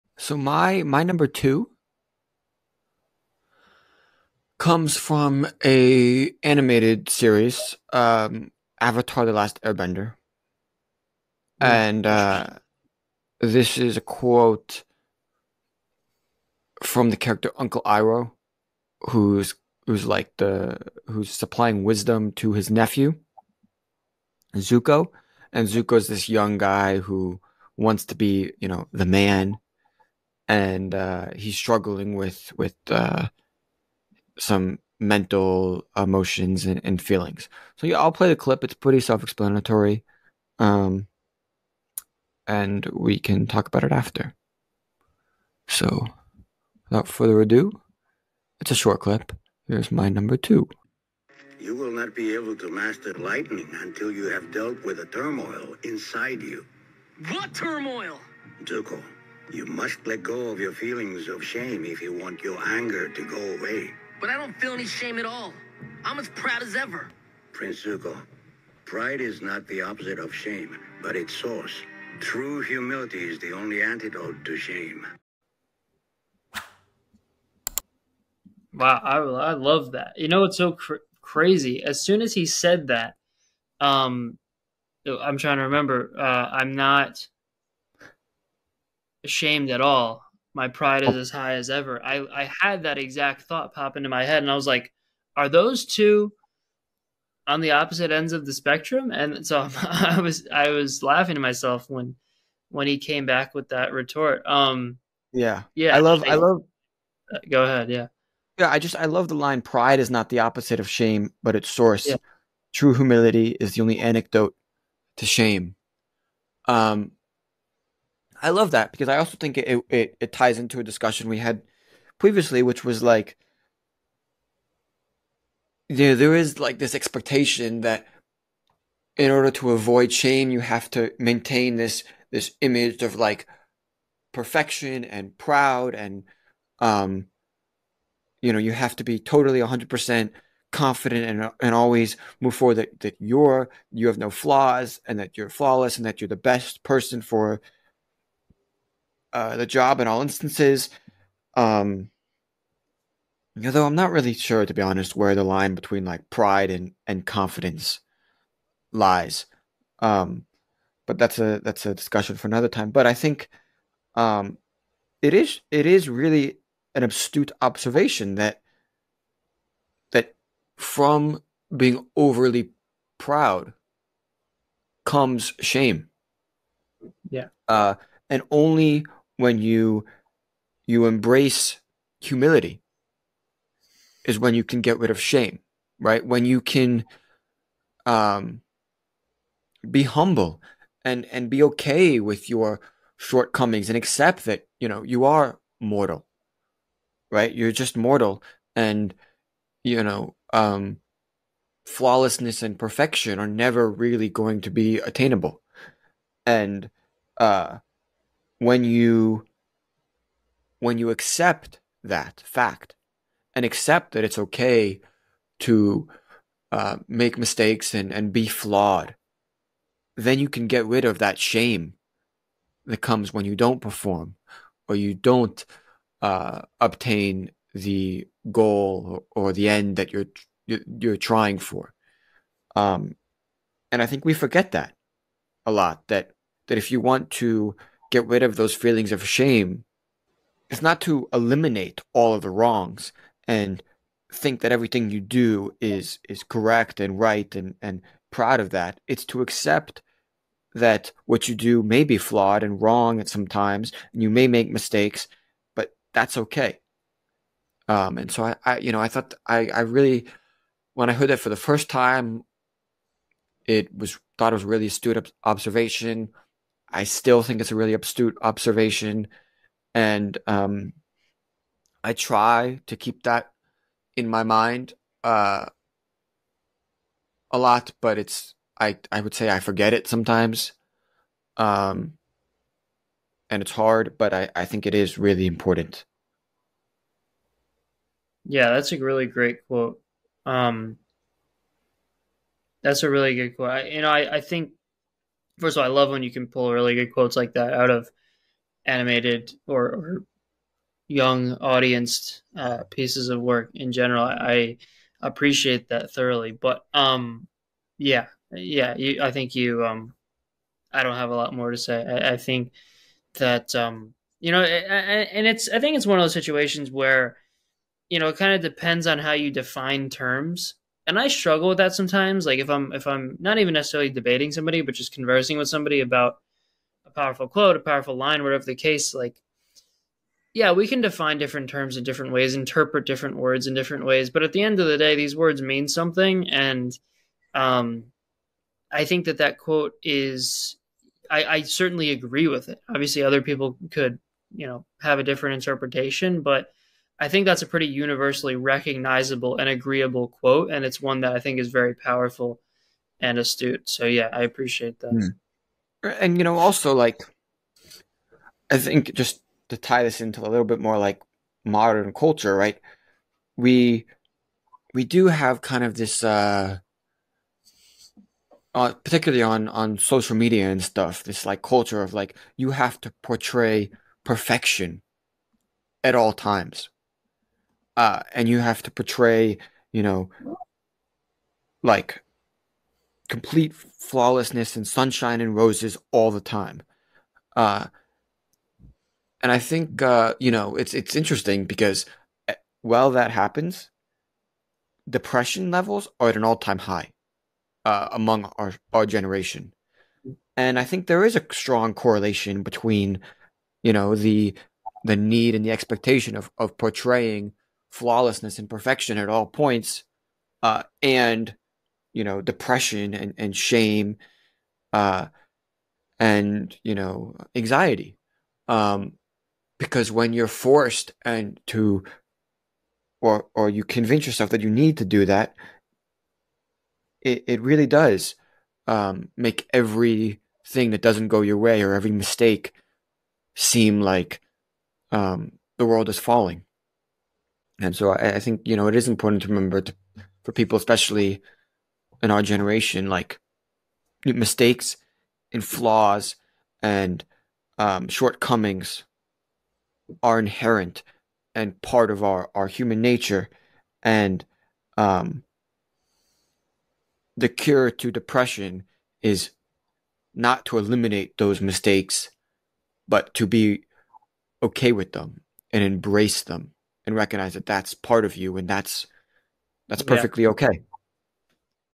How To No Longer Feel Ashamed?
In this clip, Tristan and I discuss how to stop feeling shameful. More specifically, we discuss how "humility is the only true antidote to shame," and how the modern structures of society make it very difficult for people to accept their own weaknesses, which leads to feelings of shame and depression.

This is a clip from the Top 5 TV Quotes and the full episode can be watched or on

and all other major podcast platforms.